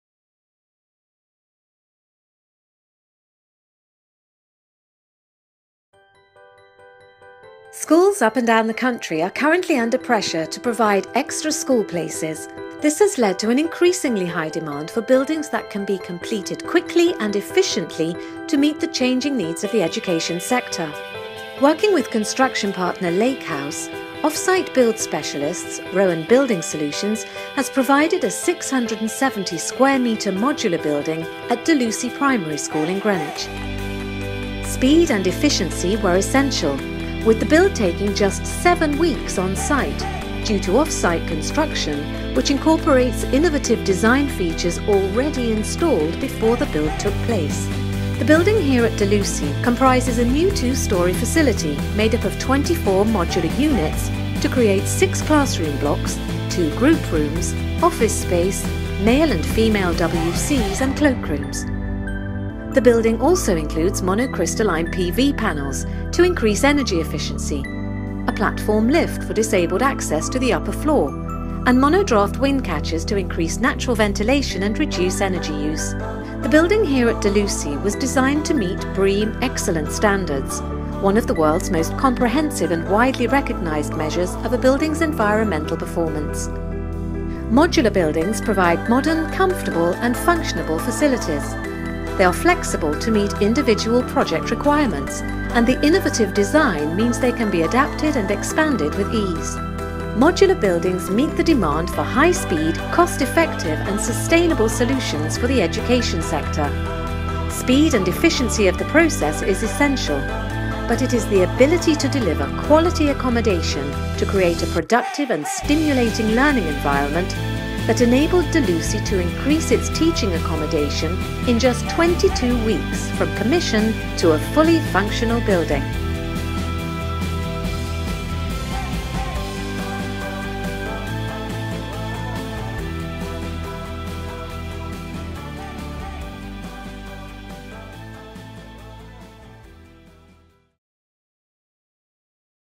Modular Classroom Facility - DeLucy Primary School
Actavo | Building Solutions was commissioned by construction contractor Lakehouse to provide a new two-story modular facility at Delucy Primary School, Greenwich.
The modular building compromises of twenty four modular units that make up a building with six classroom blocks, two group rooms, office space, male and female WC’s and cloak stores. Included in the project design and construction were internal and external staircases and additional external WC’s adjacent to the playground.
The offsite construction included mono-crystalline PV panels to increase energy efficiency, a platform lift for disabled access to the upper floor and mono-draft wind catchers to increase natural ventilation and reduce energy use.
The project was finished in coloured Trespa cladding to give it a distinctive look and was designed to meet BREEAM ‘Excellent’ standards.